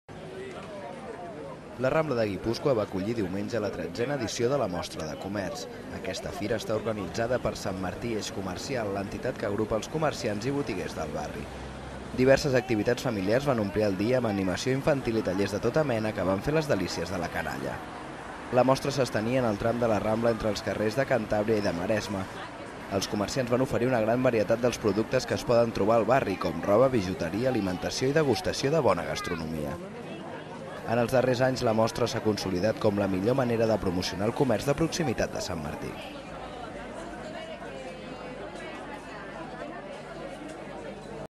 2012 05 20 Mostra Comercial Guipuscoa
La Rambla de Guipúscoa va acollir diumenge la 13ena edició de la mostra de comerç. Aquesta fira està organitzada per Sant Martí Eix Comercial, l’entitat que agrupa als comerciants i botiguers del barri. Diverses activitats familiars van omplir el dia, amb animació infantil i tallers de tota mena, que van fer les delícies de la canalla.